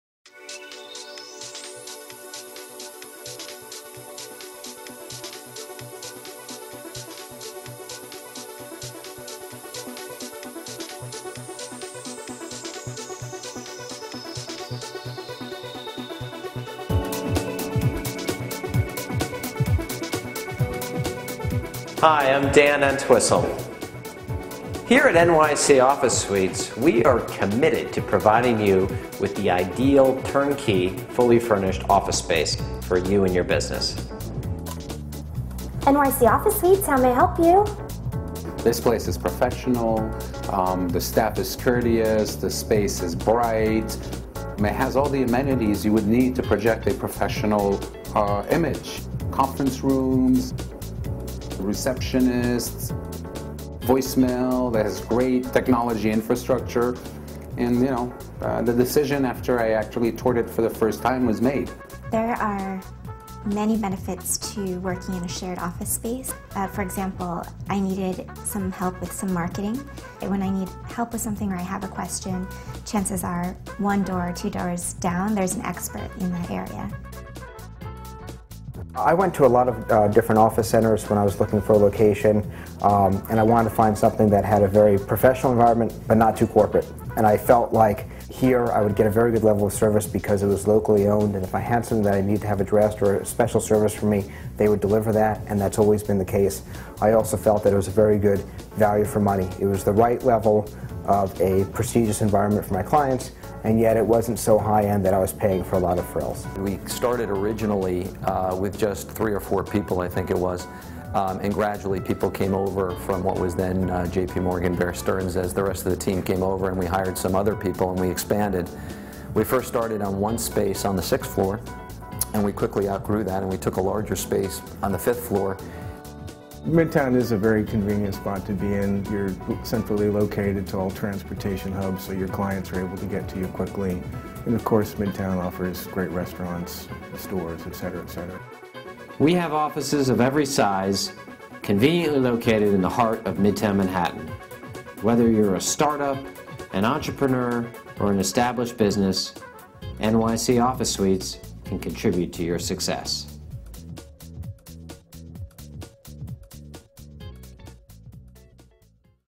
NYC Office Suites: Turn-key Flexible Office Space in Midtown Manhattan
NYC Office Suites provides turn-key office rental space with flexible rates and terms. Our three mid-town Manhattan locations offer on-site management and executive concierge services to support your business. for more information.